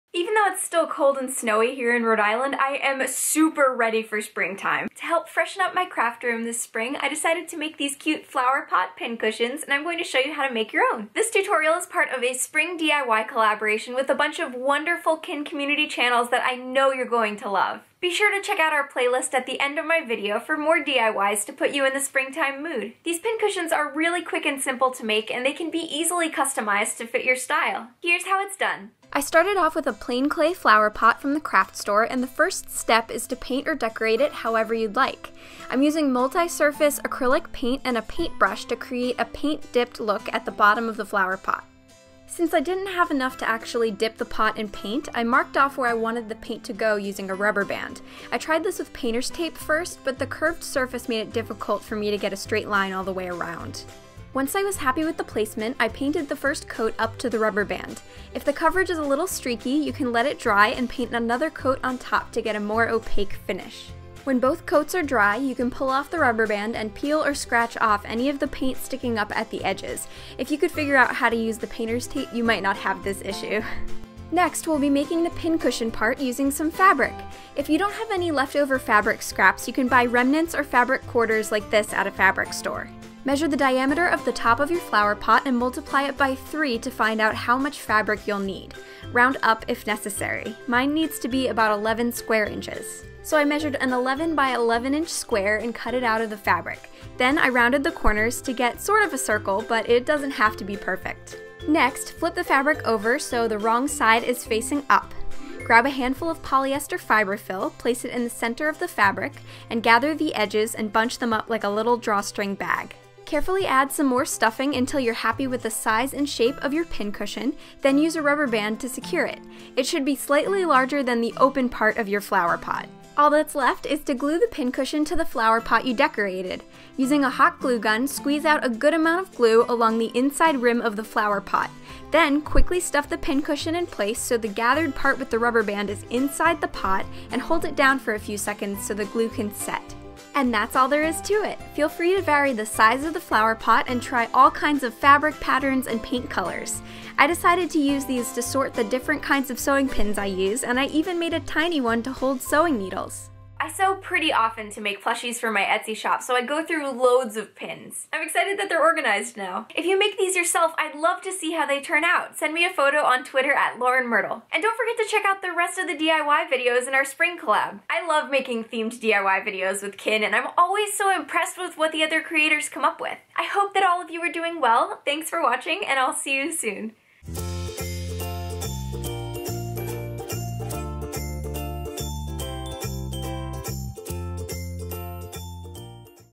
Flower Pot Pincushion Tutorial: Spring DIY
Materials you'll need for this project:
-A small clay flower pot
-Multisurface acrylic paint and a paintbrush (or other method of decorating your flower pot)
-Rubber bands
-Small piece of fabric
-Ruler
-Pencil
-Fabric scissors
-Hot glue gun
-Sewing pins

☁ Follow me @laurenfairwx on social media!

☁ Online Stores